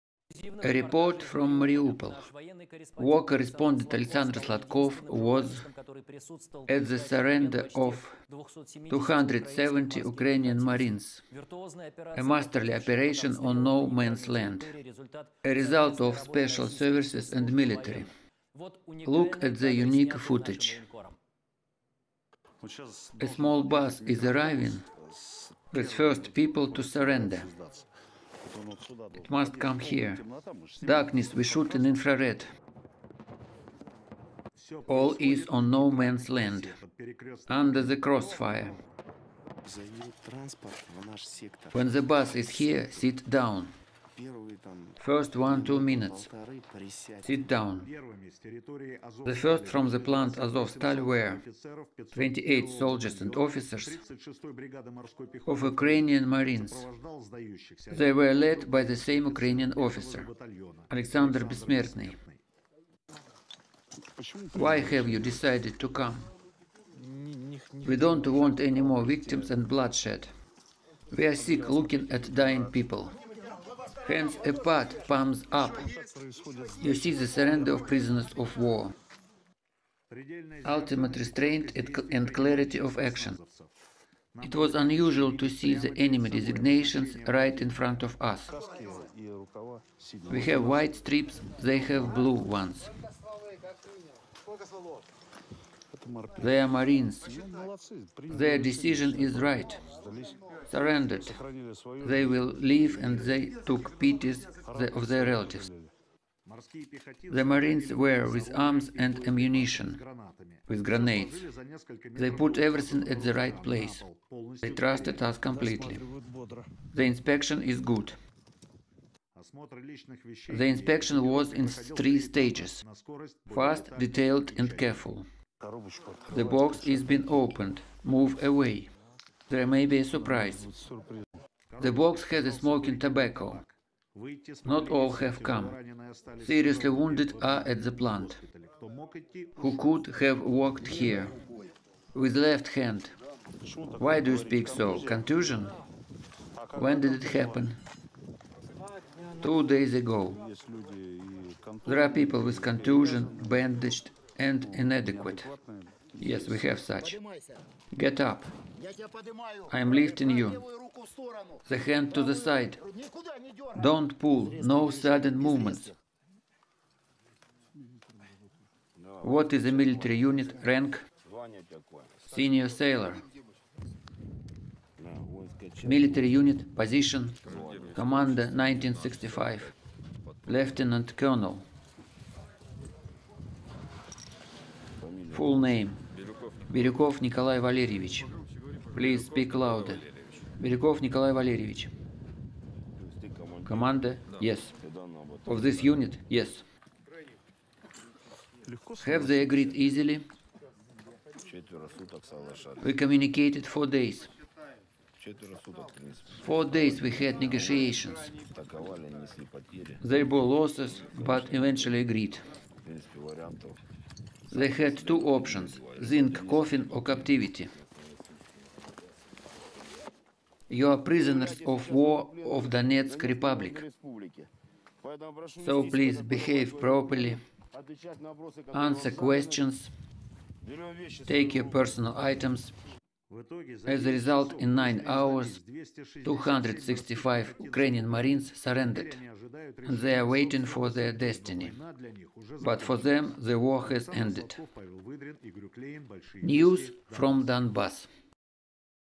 Surrender of Ukrainian marines: exclusive report from Mariupol
The Marine Corps Battalion of the Armed Forces of Ukraine in Mariupol laid down its arms.  Why was such a decision made and what did those who surrendered say?

Exclusive reportage from Mariupol.  VGTRK war correspondent Oleksandr Sladkov became the only journalist who was present at the surrender of almost 270 Ukrainian marines.  The virtuoso operation, which took place under fire on neutral territory, is the result of the joint work of the Russian special services and the military.  Unique shots taken by our military correspondent.